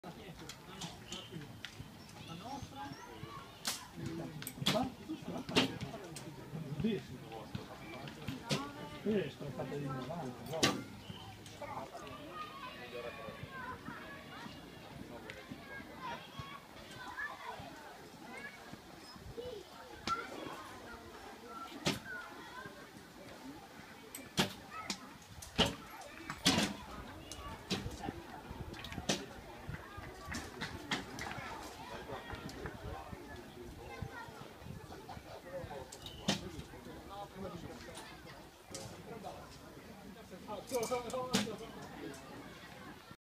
Tiro con l'arco, l'oro olimpico Marco Galiazzo a Chieri
Grande festa dello sport a Chieri con l'esordei femminile di basket, ma soprattutto col torneo interregionale che ha visto tra gli sfidanti ieri David Pasqualucci ed oggi l'oro olimpico Marco Galiazzo.